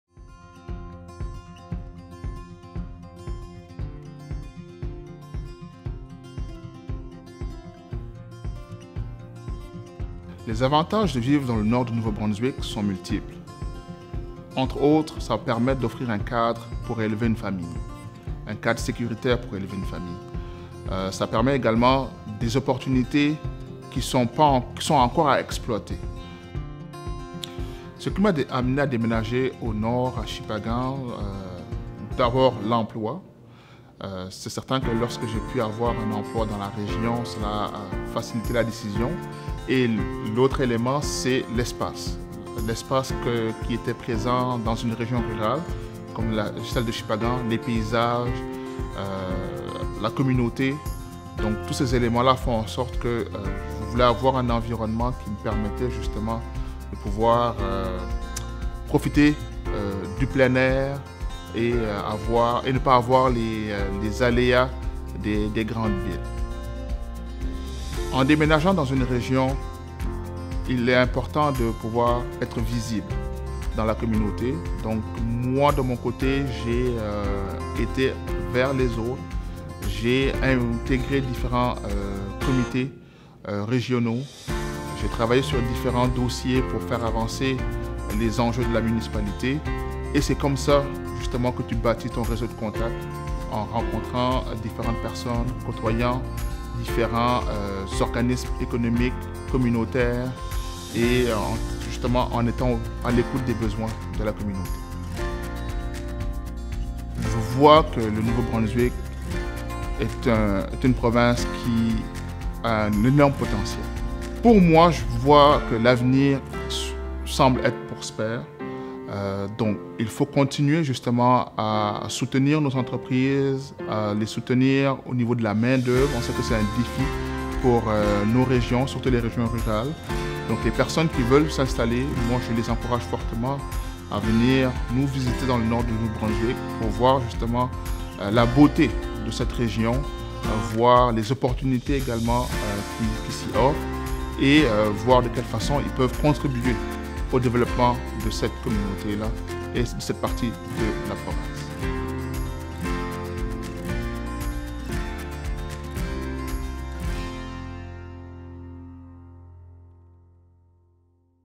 A New NB Perspective | Une nouvelle perspective pour le Nouveau-Brunswick
Born in the Ivory Coast and raised in Senegal, Kassim Doumbia arrived in New Brunswick in 2000 as an international student at The Université de Moncton before settling in Shippagan with his wife in 2007.
M. Kassim Doumbia, qui est né en Côte d’Ivoire et qui a grandi au Sénégal, est arrivé au Nouveau-Brunswick en 2000 en tant qu’étudiant étranger à l’Université de Moncton. Il s’est installé avec sa femme à Shippagan en 2007.